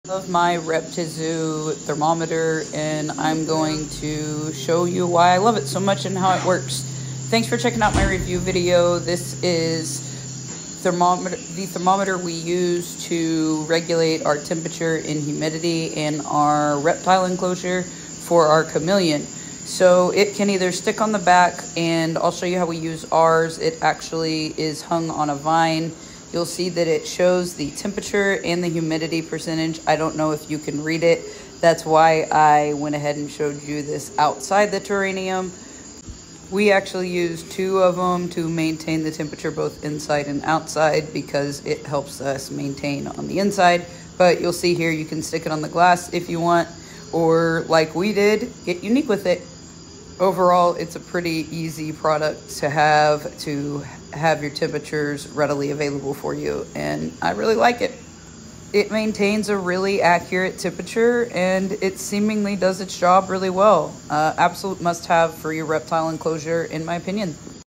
REPTIZOO Terrarium Thermometer Hygrometer Honest Review
Looking for a thermometer to keep an eye on the temperature inside your terrarium? Look no further than the REPTIZOO Terrarium Thermometer! This affordable and easy-to-use device is perfect for reptile owners of all levels of experience. Watch this video to see my honest review of this great product!

REPTI ZOO Reptile Terrarium Thermometer Hygrometer Digital Display Pet Rearing Box Reptiles Tank Thermometer Hygrometer with Suction Cup